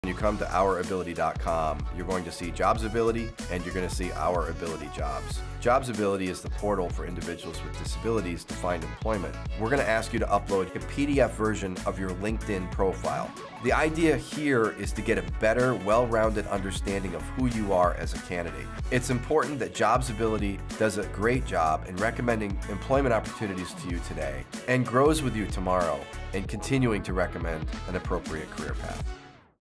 Using Generative AI with Our Ability
Generative AI with Our Ability for people to match to open employment opportunities. We are people with disabilities, building a web empire to assist people with disabilities in finding employment and accessibility.